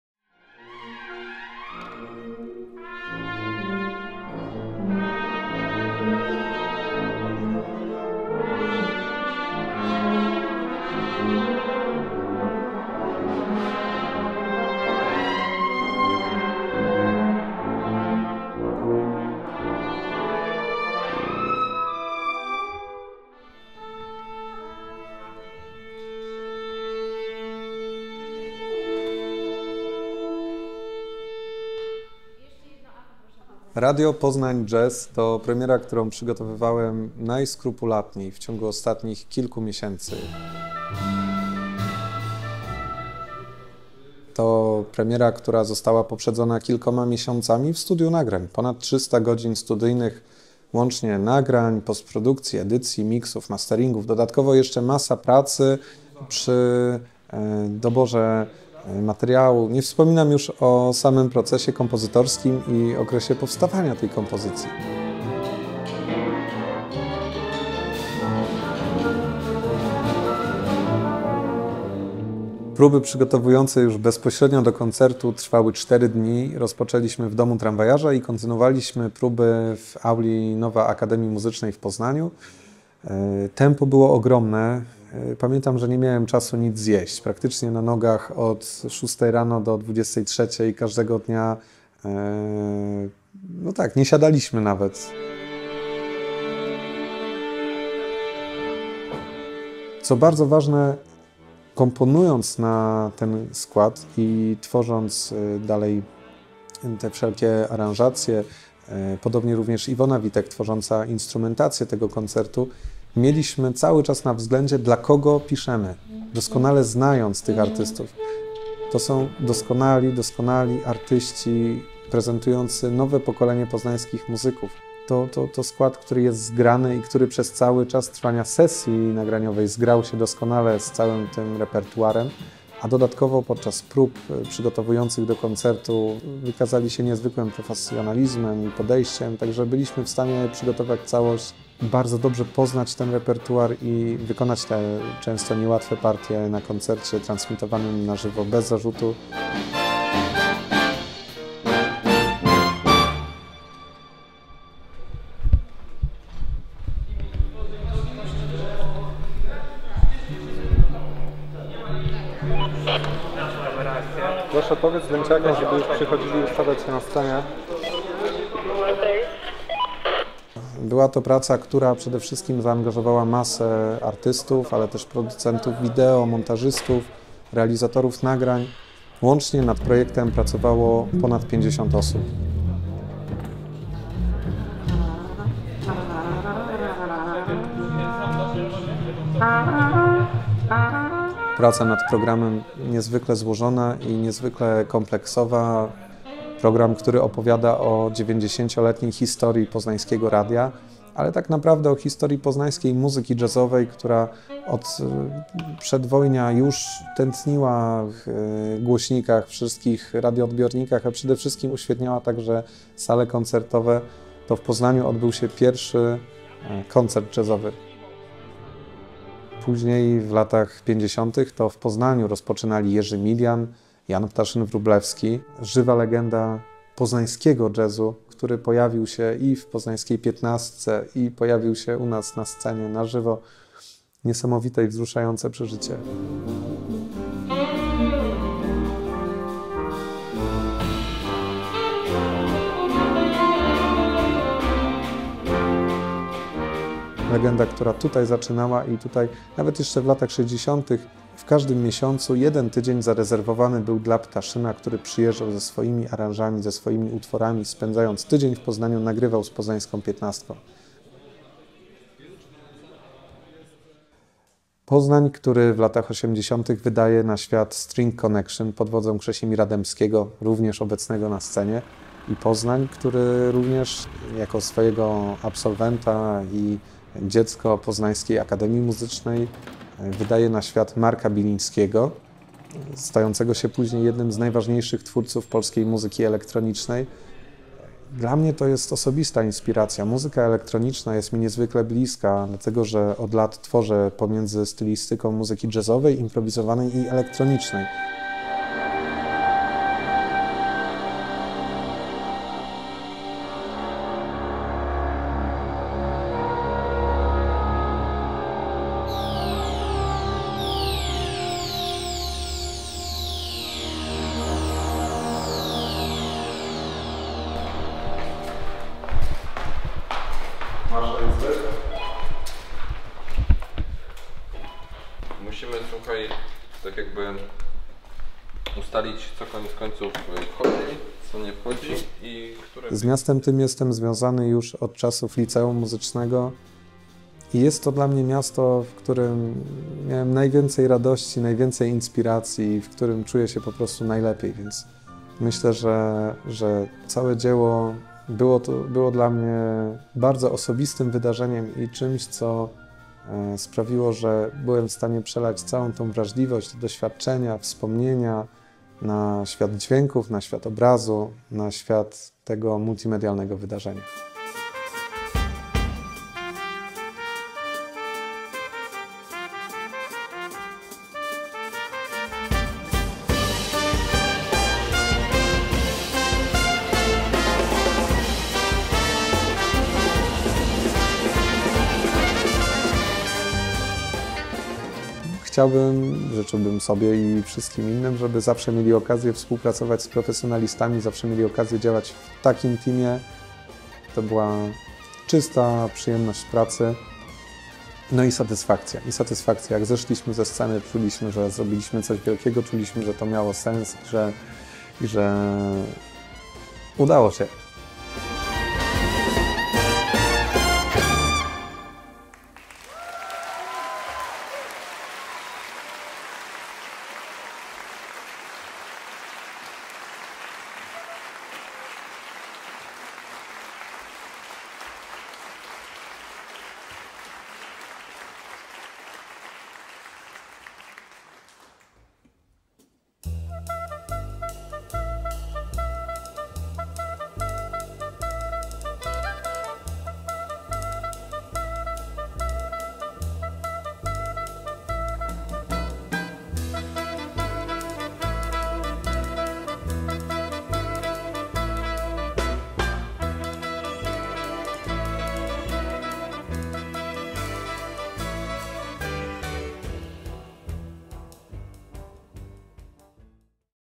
Making of RADIO, POZNAŃ, JAZZ (english subtitles)
Reportaż o kulisach koncertu „Radio, Poznań, Jazz” zrealizowanego w ramach jubileuszu 90-lecia Radio Poznań. O projekcie opowiada kompozytor i reżyser tego przedsięwzięcia Maciej Fortuna.